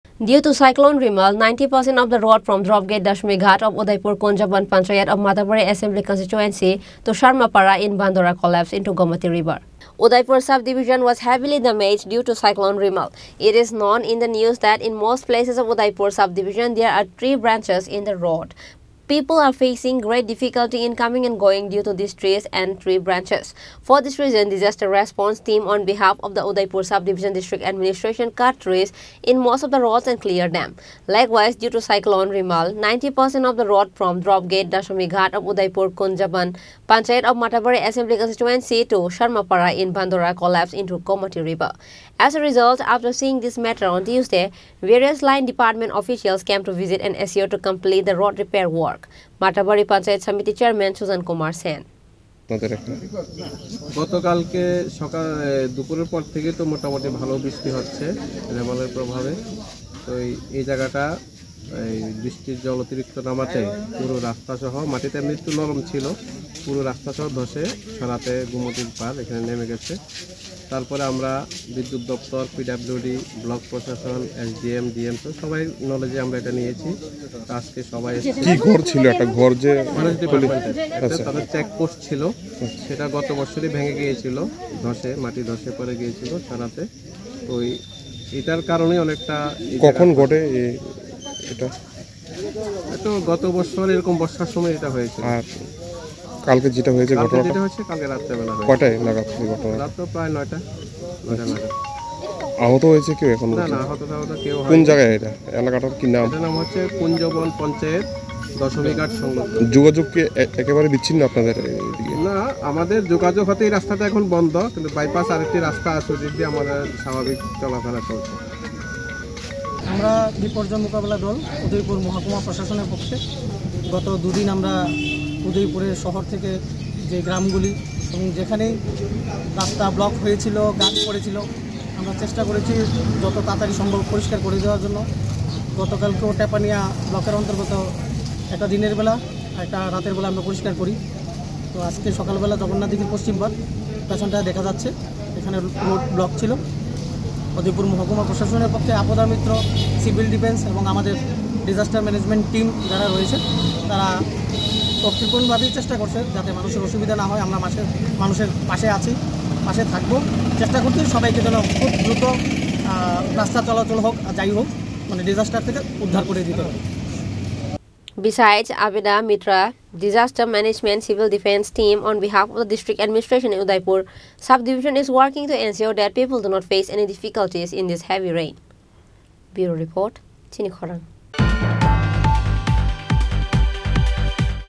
90 percent of road of udaipur Sub Division has collapsed due to rain
🟢Due to Cyclone Rimal, 90 percent of the road from Dropgate Dasami Ghat of Udaipur Kunjban Panchayat of Matabari Assembly Constituency to Sharmapara in Bandora collapsed into Gomati river.